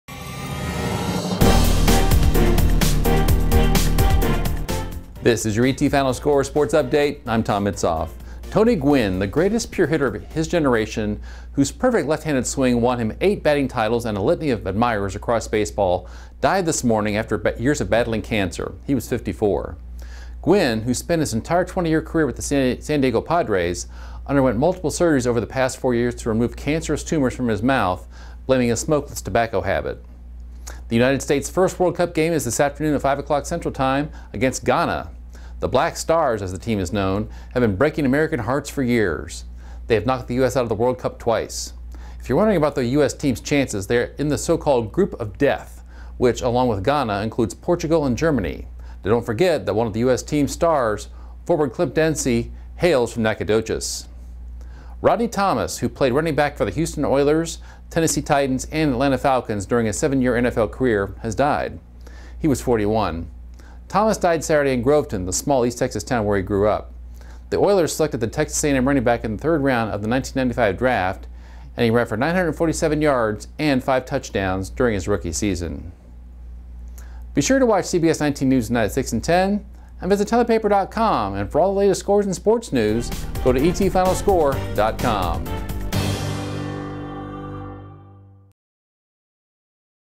U.S. opens World Cup play against rival Ghana today
ETFinalScore.com Sports Update video for June 16, 2014